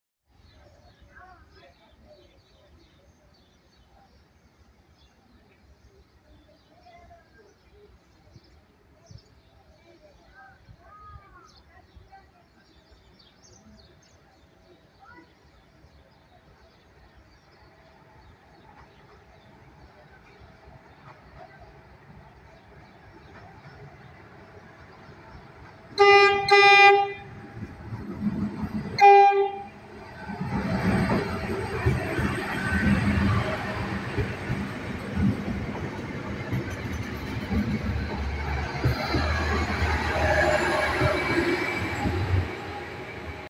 ECCO L' IBRIDO! - InterCity 562 HTR412 PER BARI!!
Da poco estinte le D445 , al via agli ibridi: HTR412, in questo filmato vediamo il transito alla stazione Omeca dell' IC 562 per Bari.

4 coppie giornaliere che collegano Reggio Calabria Cle con Bari Cle e Lecce.